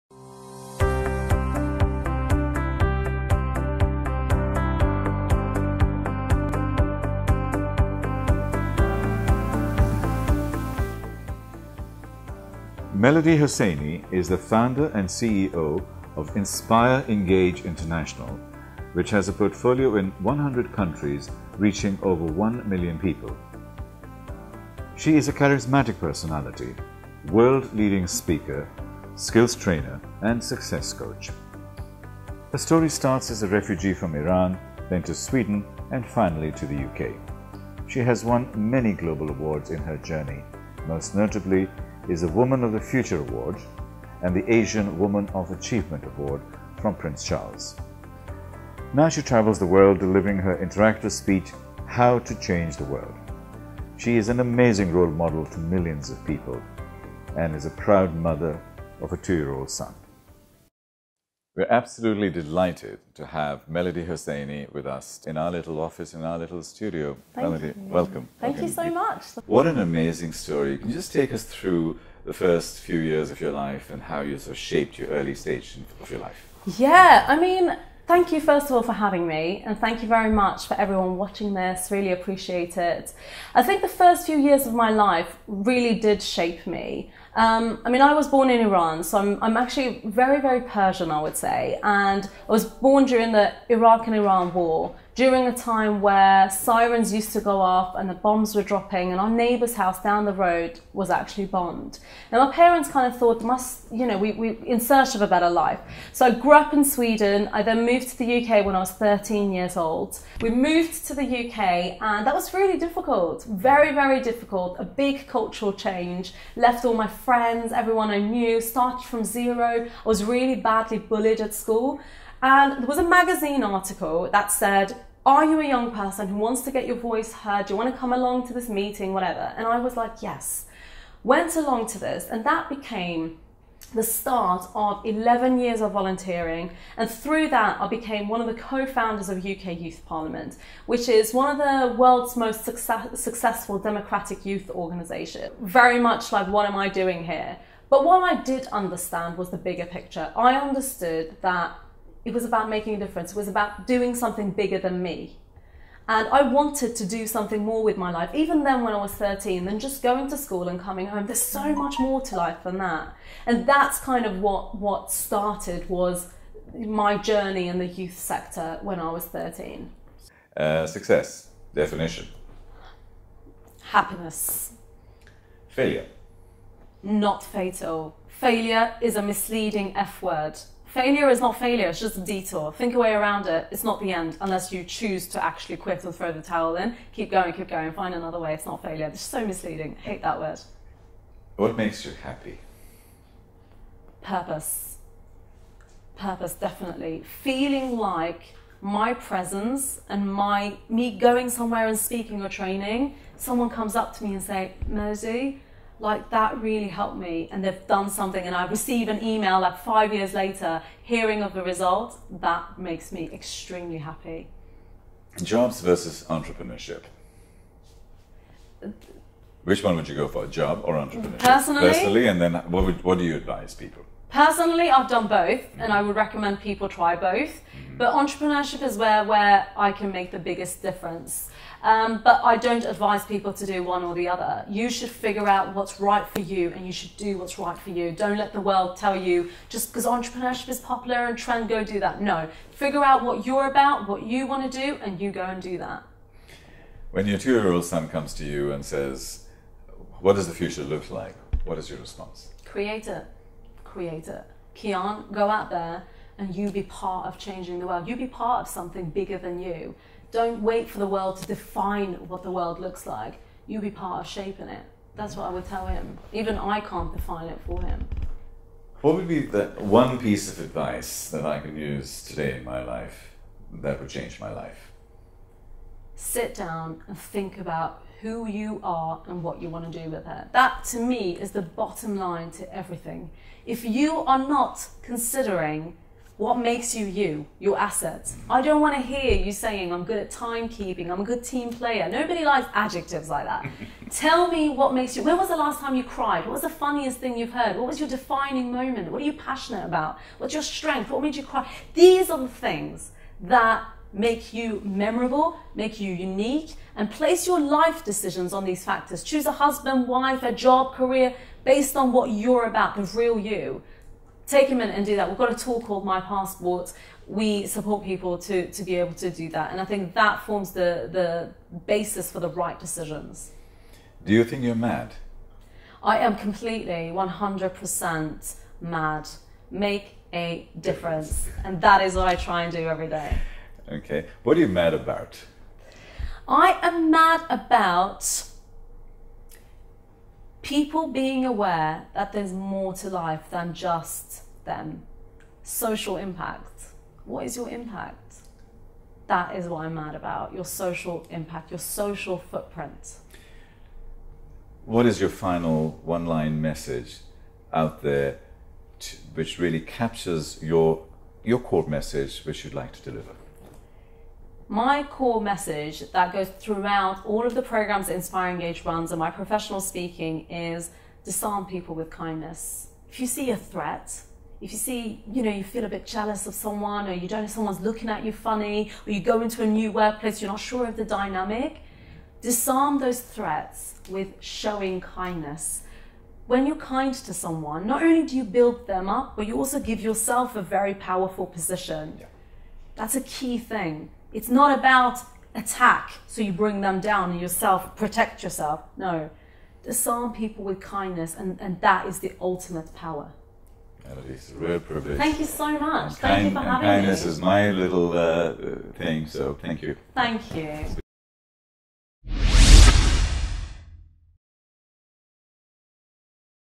Interview with Melody Hossaini, Founder & CEO of InspirEngage International
Welcome to this episode of our Bayt Leadership Series. Melody Hossaini, the Founder and CEO of InspirEngage International, started working at the age of 13 and today has reached over 1 million people in over 100 countries. She helps individuals to find their purpose, their personal brand and how to develop essential skills to find happiness and create impact. In this video, she tells MAD Talks founder Tariq Qureishy about her life story - how she created her business, how she immigrated to the UK from Iran, about volunteering, motherhood and finding purpose in life through work. She tweets at @Melody_Hossaini.